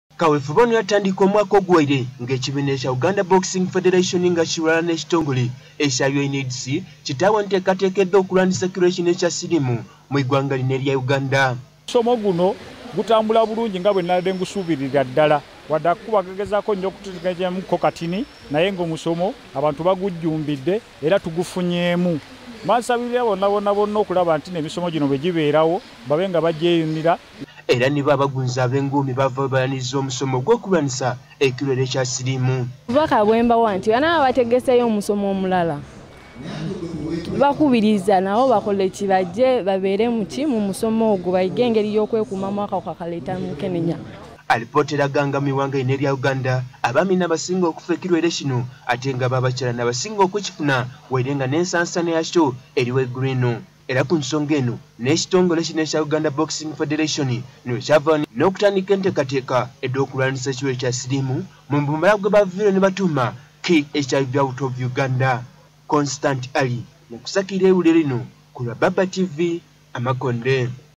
Abebikonde benigiire mukulwanisa akawuuka ka mukenembya
Kawefumbe we kibiina ekitambuza omuzaanho gwe’bikonde mu
gwanga ekya Uganda boxing federation okulwanisa akawuka ka
mukenembya ekaaja mu maiso era olunaku olwa leero basiibye
Kiwatule ku Rise and shine boxing club.
Omusasi waffe abaireyo.